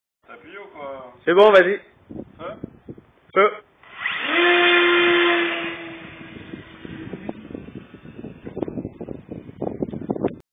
Multiplex Blizzard - Overpowered 4S Chair Launch !
Relax, take it easy! Handlaunching my overpowered Blizzard in a relaxing mood. Enjoy ! Power is the key ! The setup is 4S. This plane is my favorite plane. Almost unbreakable. OVERPOWERED. I'm gonna try to put more and more ! More to come.